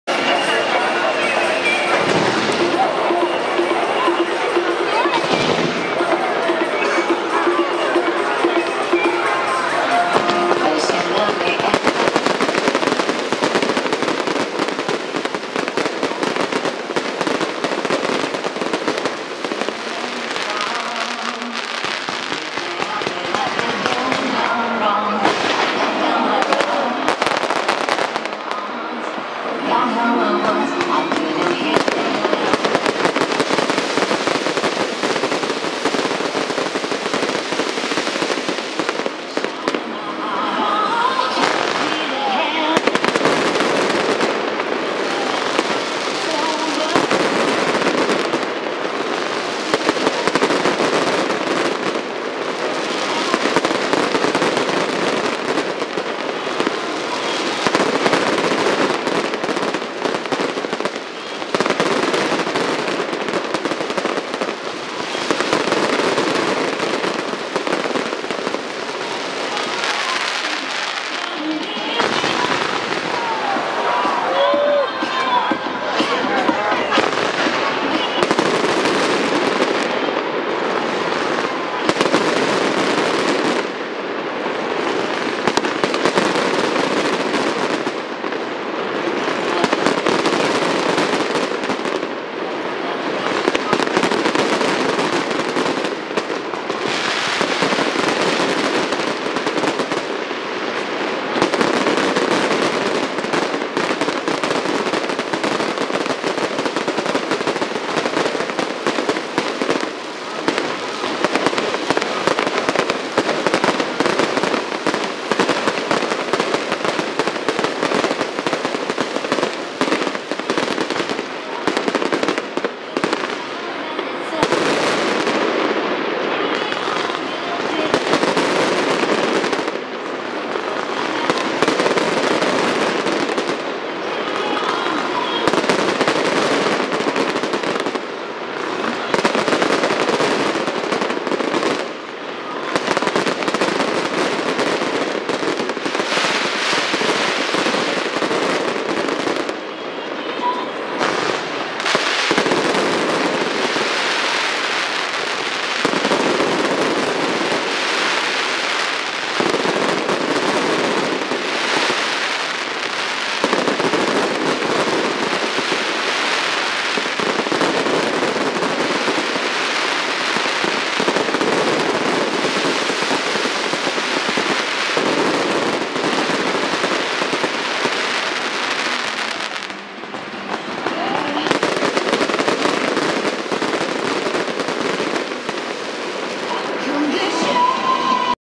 2017.02.01 2017高雄燈會藝術節 愛河煙火秀 [秀影365]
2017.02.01 高雄愛河 丁酉年 農曆正月初五